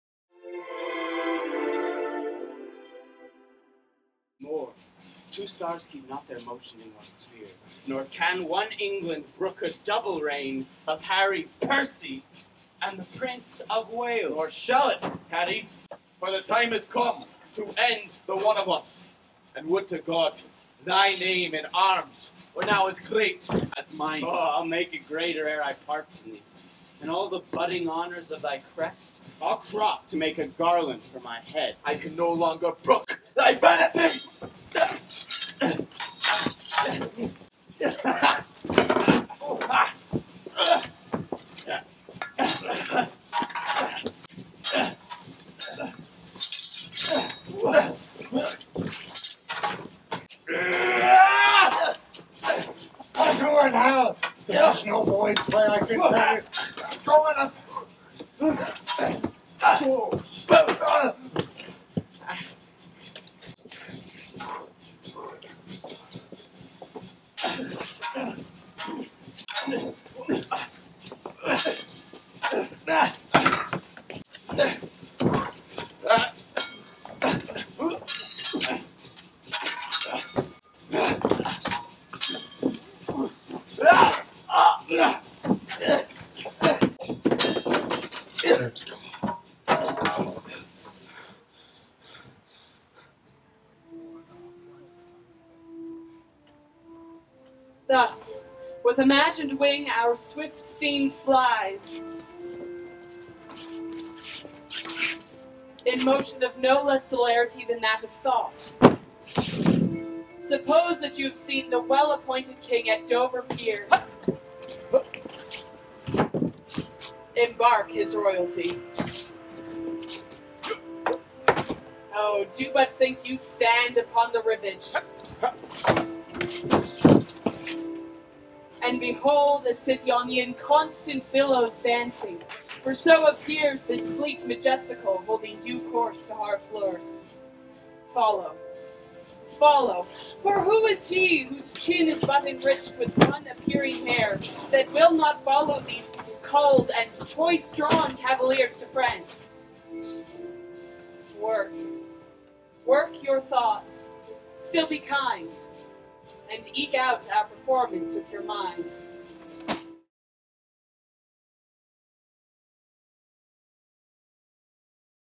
Repercussion Theatre rehearses Harry The King
Repercussion Theatre rehearses Harry The King as they prepare to take the play to Montreal area parks for 26 performances. The company rehearsed the play in Pierre Elliott Trudeau Park in Côte Saint-Luc on Thursday July 3rd 2014. (Allen McInnis / THE GAZETTE)